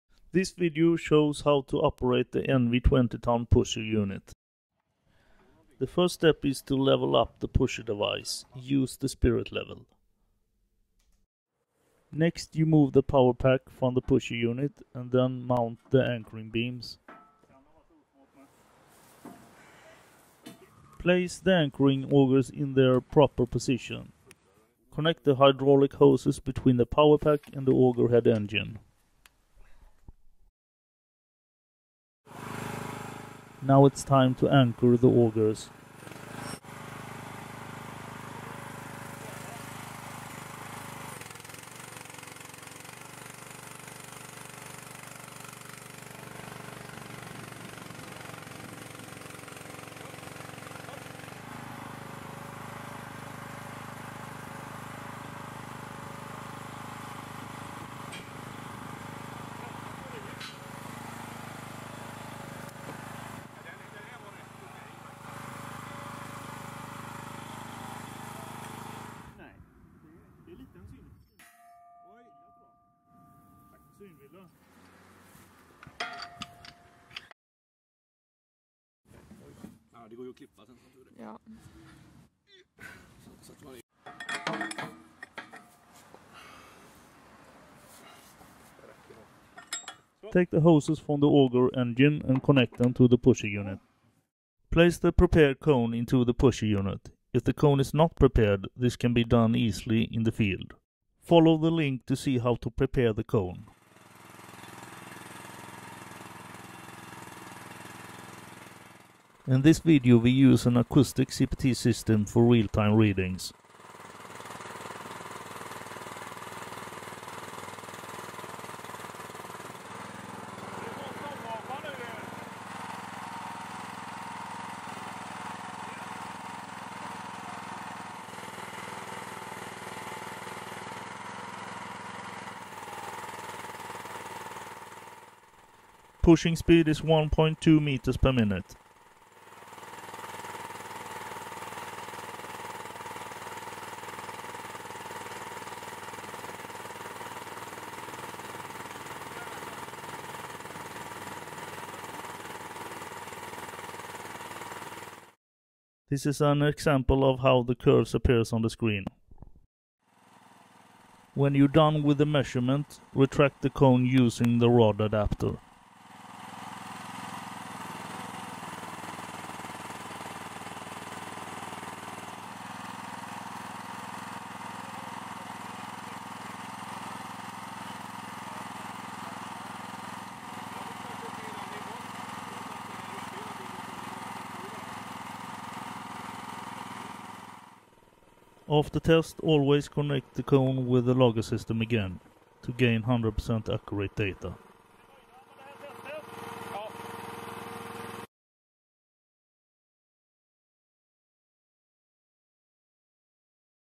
CPT (Cone Penetration Test) - Envi 20 ton Pusher unit
This video explains how to setup and perform a CPT sounding, using the stand-alone penetrometer pusher unit, and the Memocone cordless CPTu probe.